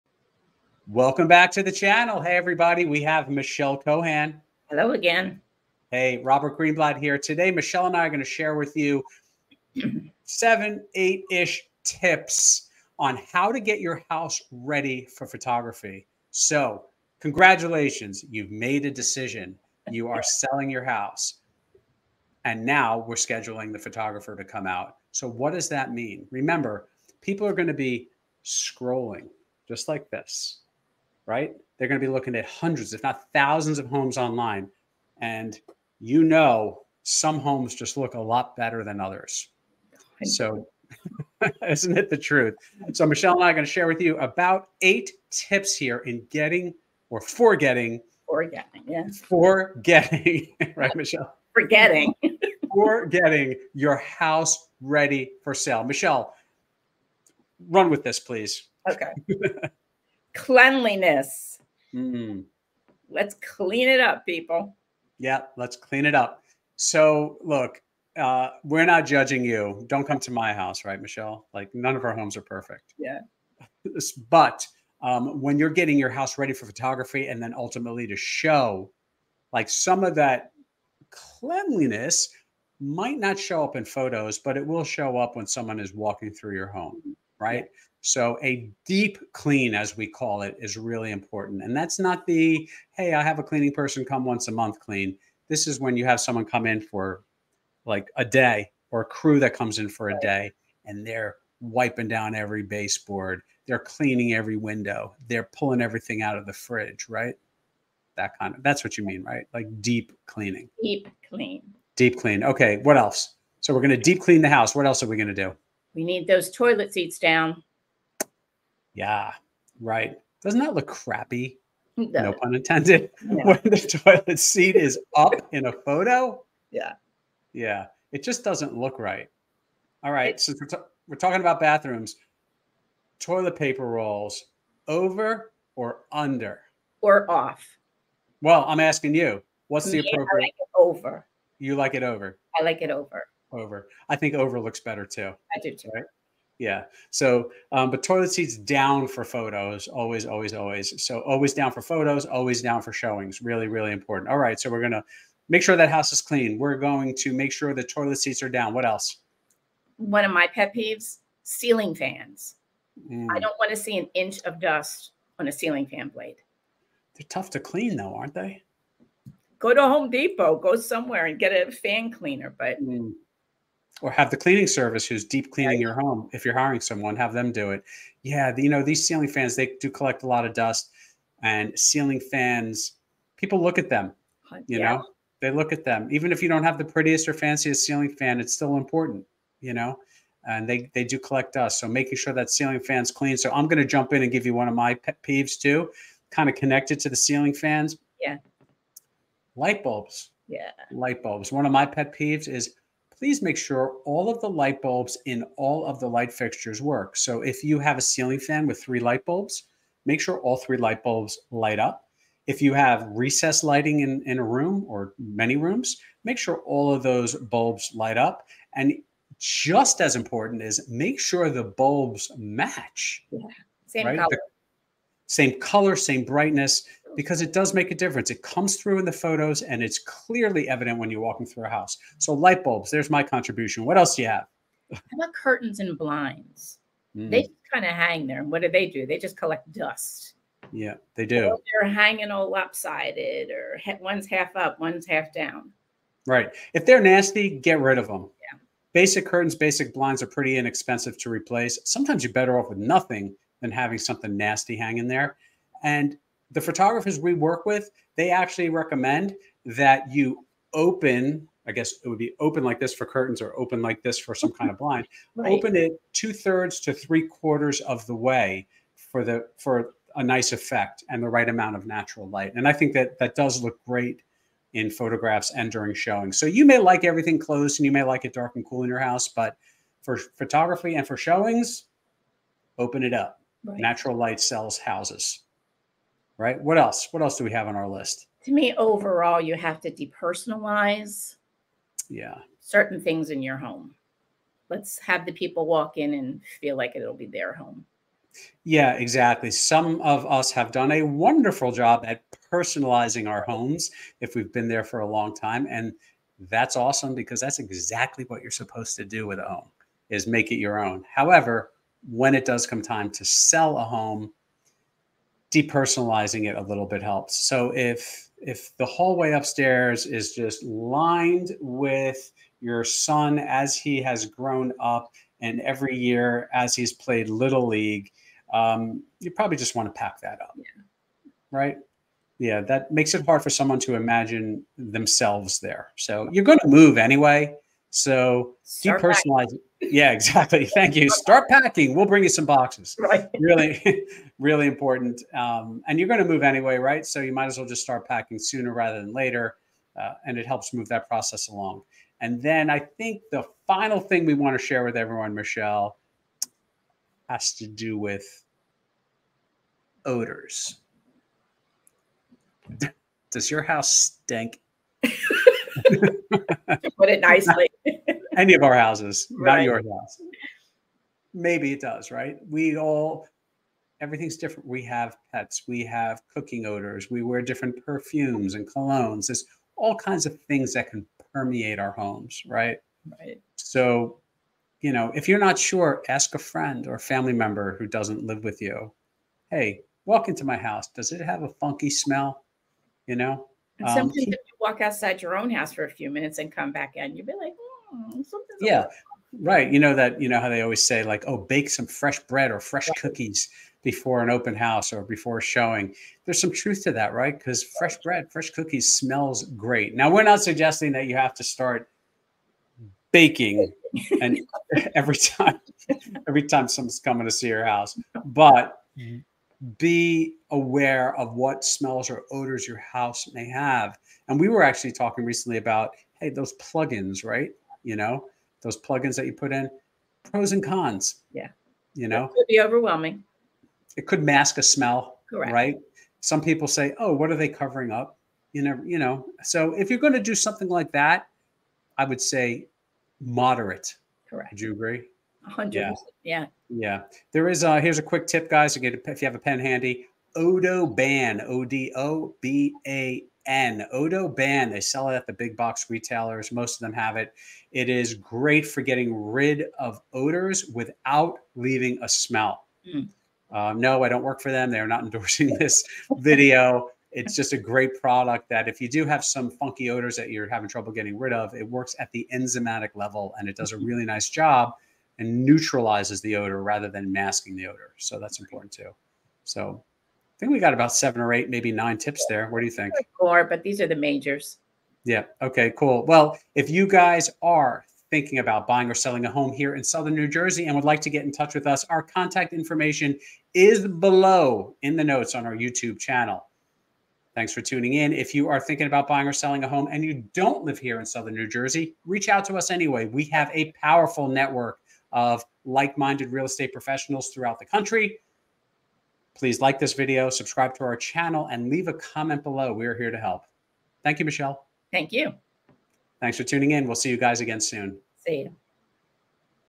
How to get your house ready for real estate photos
Join us as we discuss some important and EASY tips to get your house ready for professional photos.

Need some help with a property in the greater Cherry Hill area of southern New Jersey?  Camden County, Burlington County, Gloucester County and surrounding areas.

Need a great agent somewhere else?  We can introduce you someone from our powerful network of excellent real estate agents.